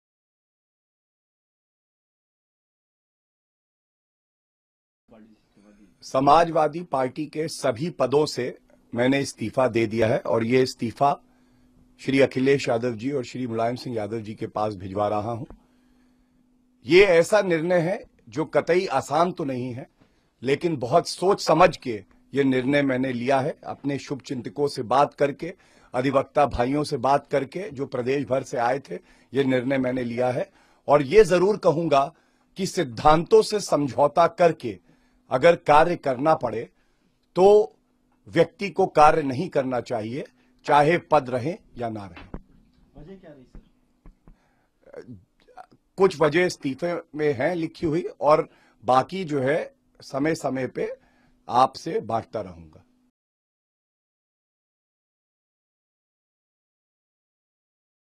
SP leader Gaurav Bhatia resigns, says didn’t want to compromise with principles
Lucknow, Feb Feb 06 (ANI): Confirming his resignation from the Samajwadi Party, Gaurav Bhatia said he did not want to compromise with his principles and that is why he took the ‘tough decision’. Bhatia further informed that he had forwarded his resignation to party patriarch Mulayam Singh Yadav and Chief Minister Akhilesh Yadav. Earlier, Bhatia resigned from the post of National President Legal Wing of the Samajwadi Party and all other posts in the party.